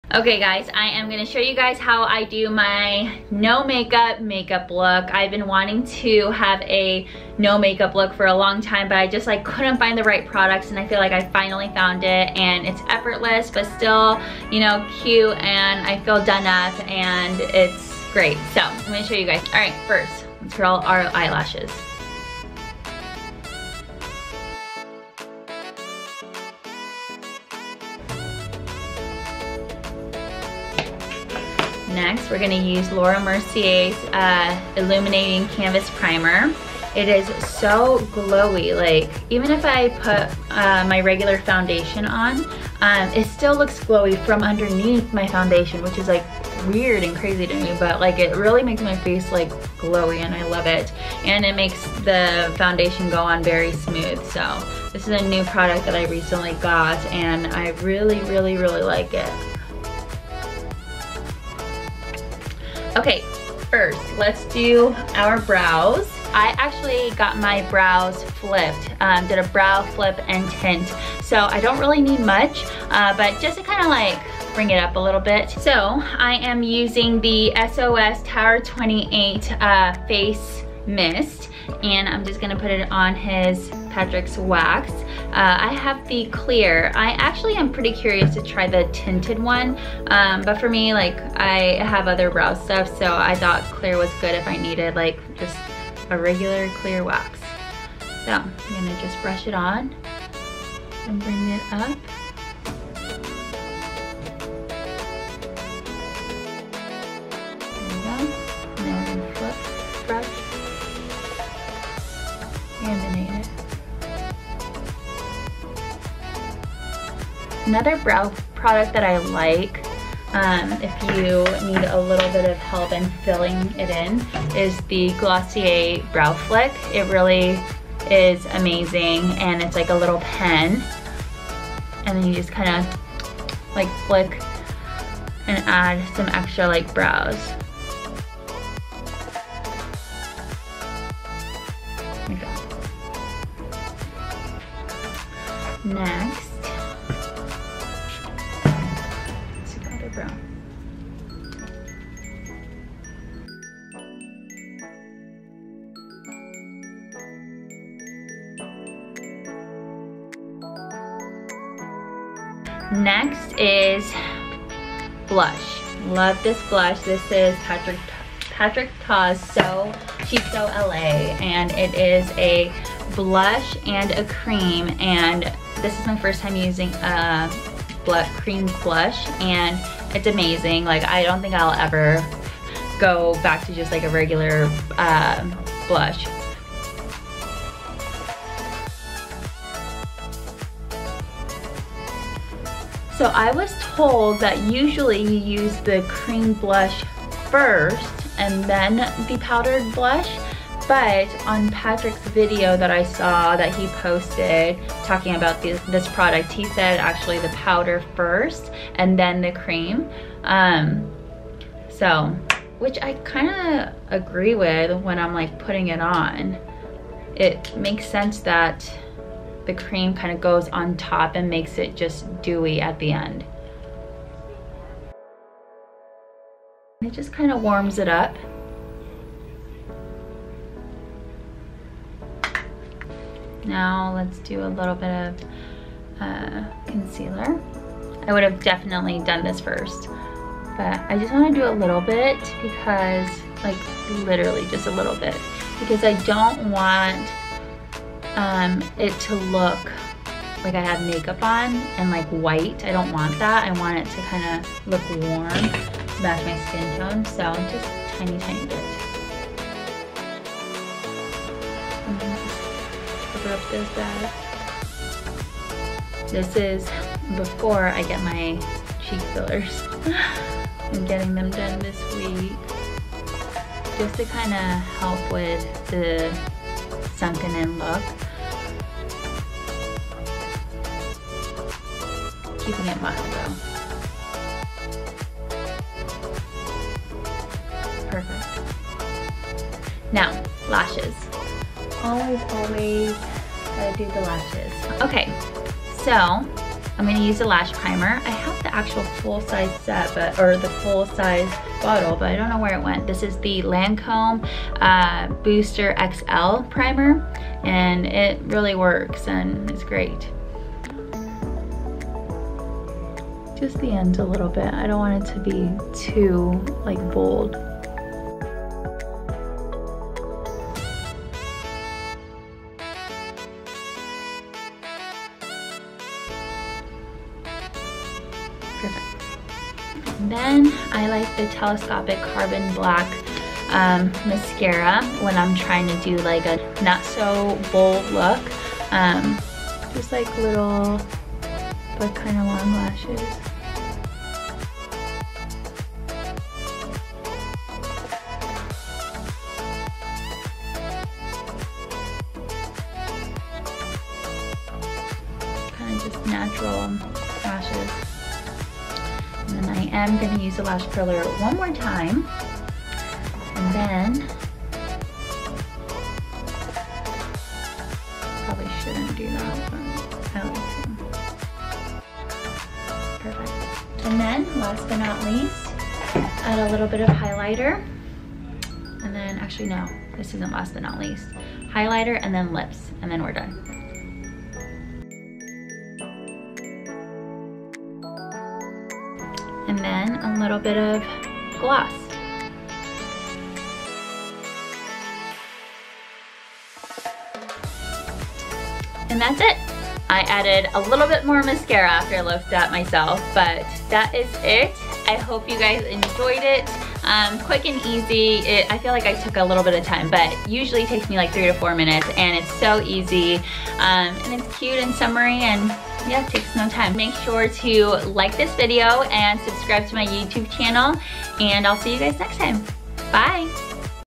My "No Makeup" Makeup Look - TAKES ME 3 MIN
A quick makeup tutorial on how I do my "no makeup" makeup look!  This look takes me about 3 min and barely any products!  Enjoy! xx